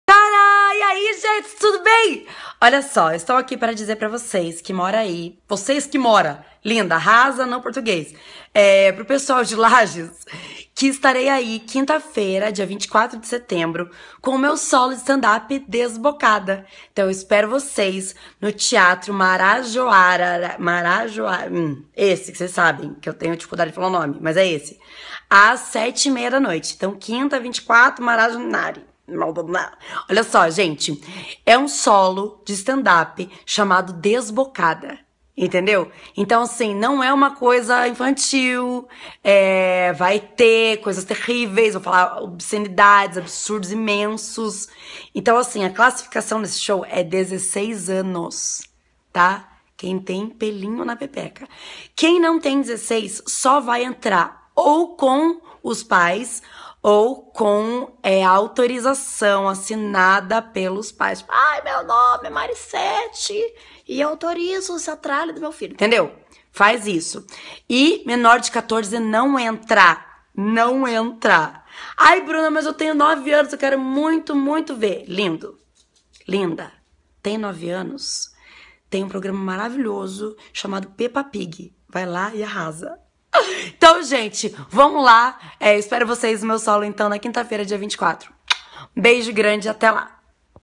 Bruna Louise convida lageanos para assistir seu Stand Comedy "Desbocada"
Finalmente em Lages um dos fenômenos da internet do momento. Bruna Louise, parceira de Kefera Buchmann no espetáculo Deixa Eu Te Contar, chega à Lages com seu stand up comedy Desbocada. Com mais de 500 mil seguidores, a comediante tem lotado todos os Teatros por onde tem passado, e por aqui não deverá ser diferente. Dia 24 de setembro às 19h30min no Teatro Municipal Marajoara. Produção local: Roger Andrade.

Os ingressos já estão sendo vendidos na bilheteria do Teatro. Lugares numerados.